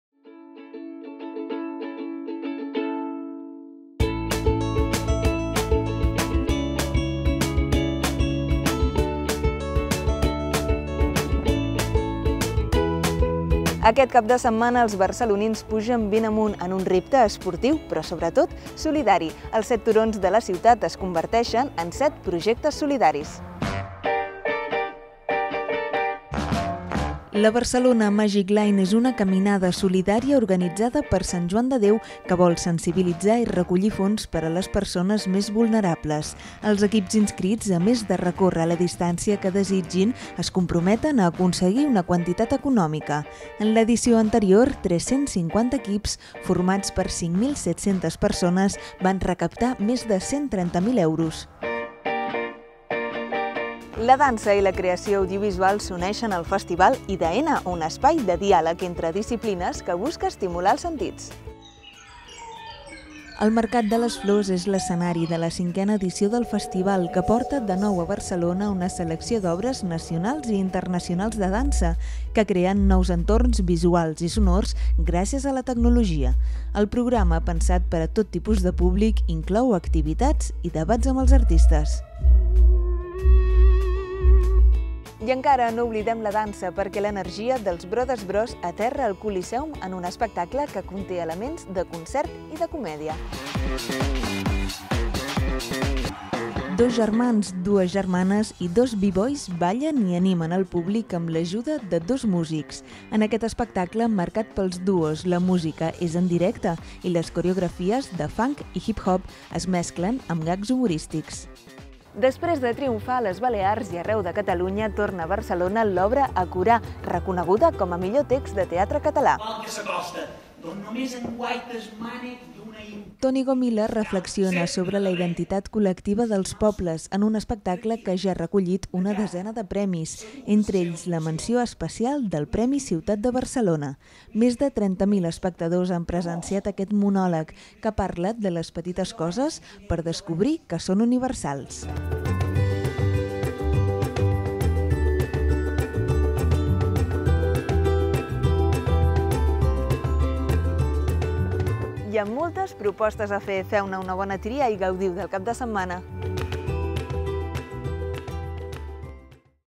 Cap de setmana: Molt més que una caminada!
La Barcelona Magic Line és un repte esportiu, però sobretot solidari en favor de Sant Joan de Déu. D’altra banda, la ciutat aplega molta dansa, amb el festival IDN i l’arribada dels Brodas Bros al Coliseum; i Toni Gomila torna a Barcelona amb l’exitosa obra de teatre Acorar. Més informació